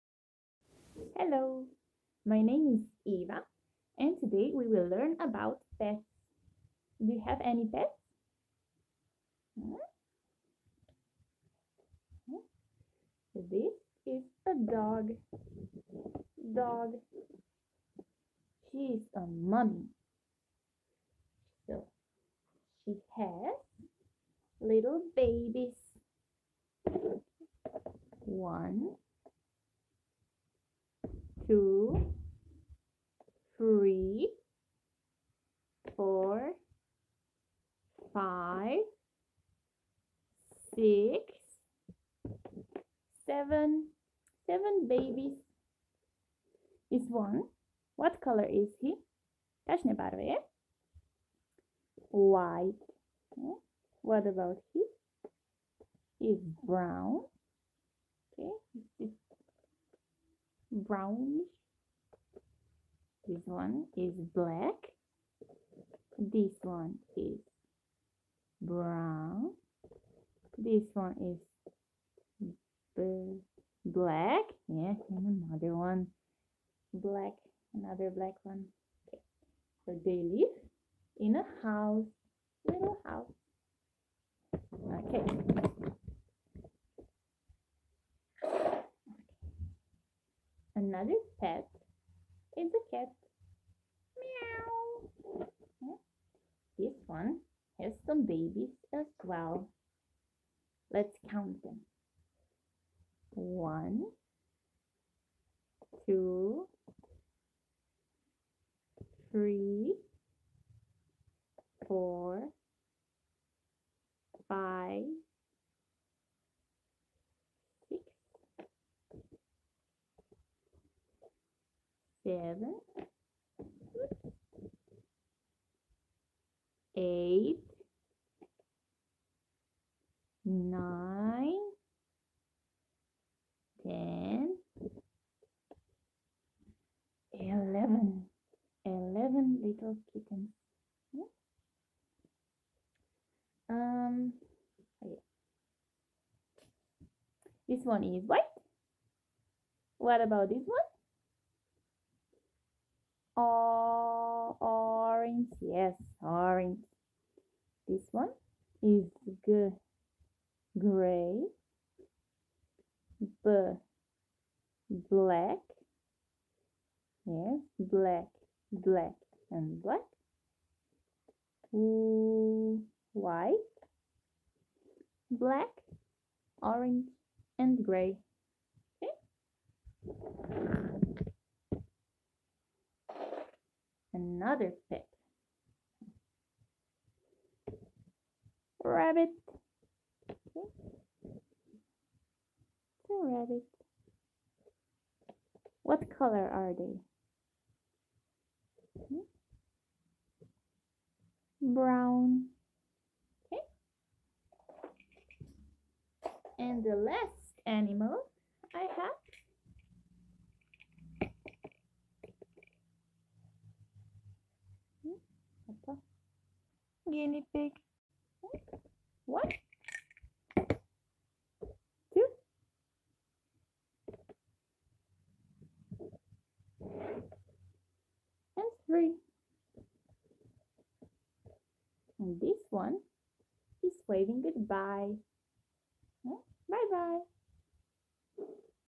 Eva's Little Pets
Teacher Eva shows you her collection of little pets. How many cats can you see?